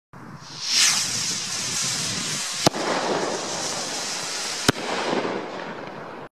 Silberhütte Donnerhall Rakete
Eine laute Knallrakete mit kräftigem Nachhall